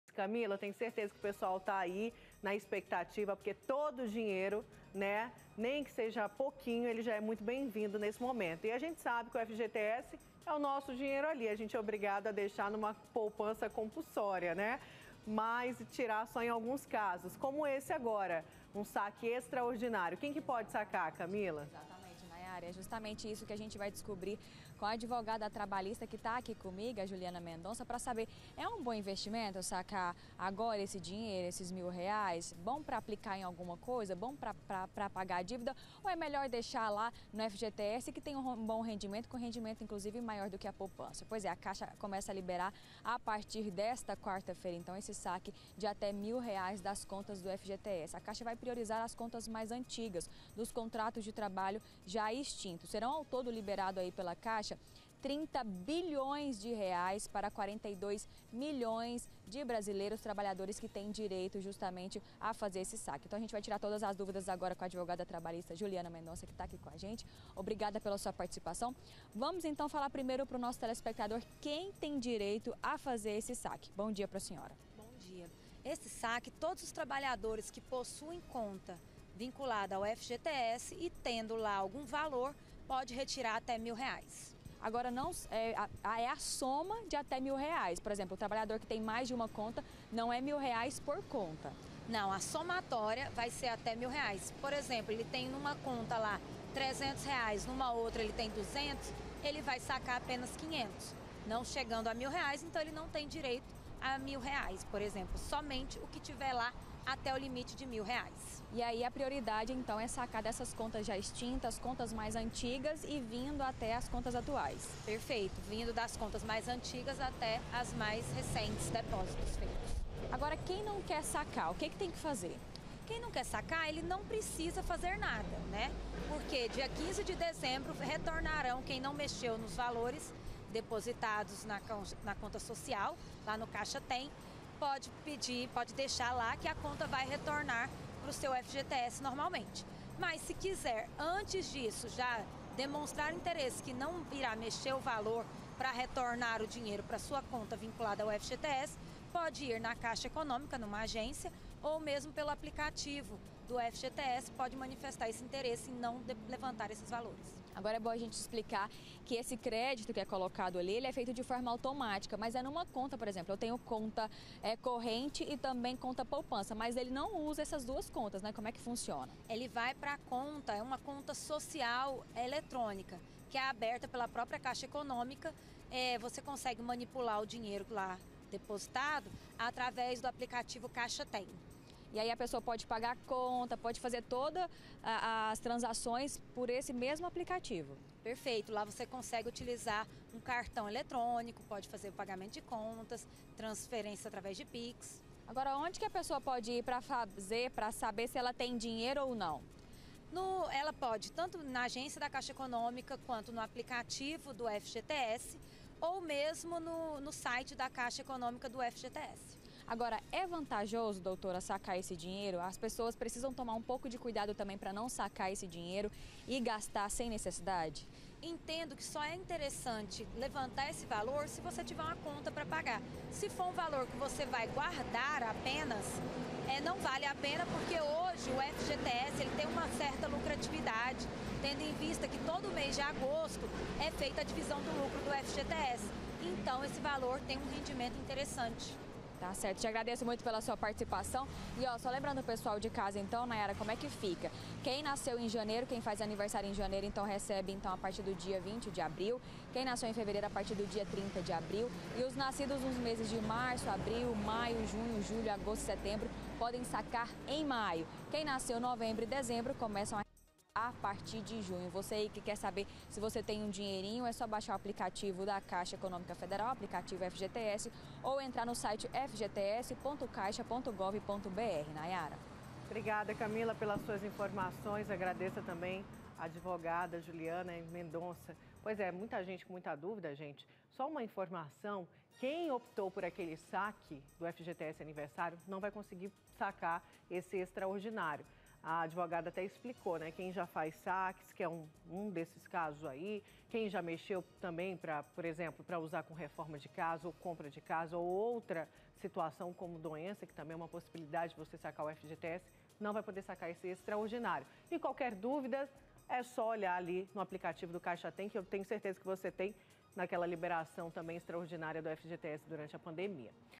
FGTS: SAQUE EXTRAORDINÁRIO COMEÇA ESSA SEMANA
COMEÇA ESSA SEMANA O SAQUE EXTRAORDINÁRIO DE ATÉ MIL REAIS DO FGTS.